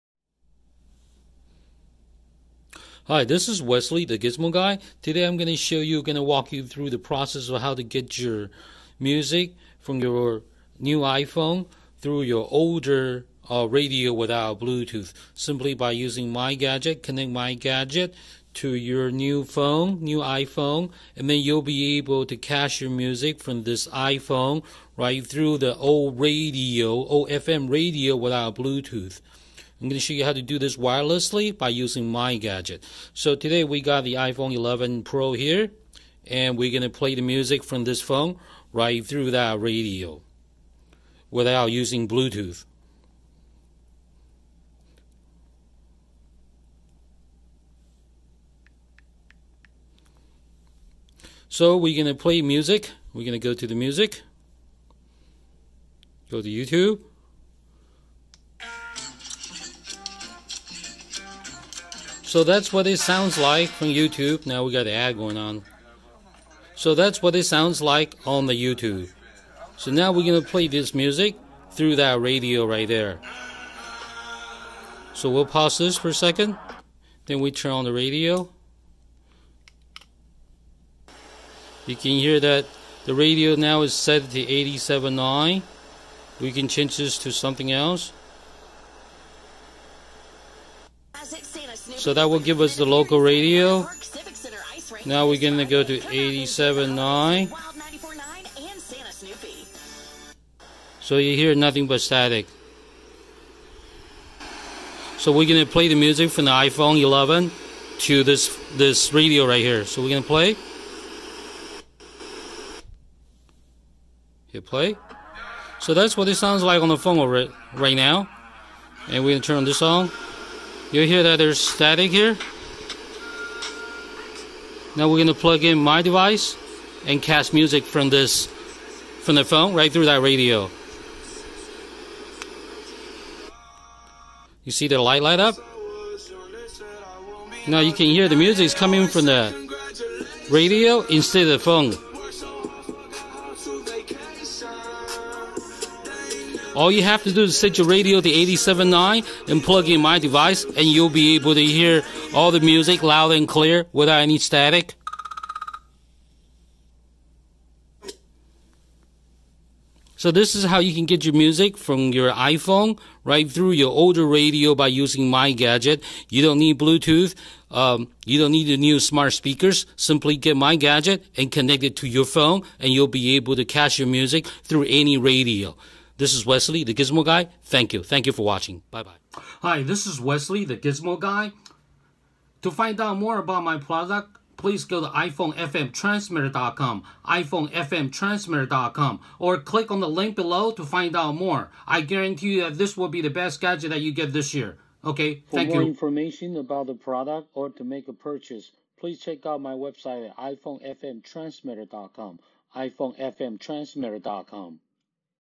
Connect iPhone to Radio ( Cast Music Thru Portable FM Radio )
Connect iPhone to Radio - Cast iPhone Music Thru Old FM Radio with No Bluetooth using Gizmo Guy Gadget.

Connect iPhone to Radio ( Cast Music Thru Old FM Radio ) Make your old car feel new again!

No Bluetooth? No Aux? No Problem! Add iPhone music to your old fm radio with cassette and CD Player to Stream Pandora, Podcast, Audiobooks from your phone thru your classic vehicle wirelessly!

Updated for 2021 for headphone-less iPhones.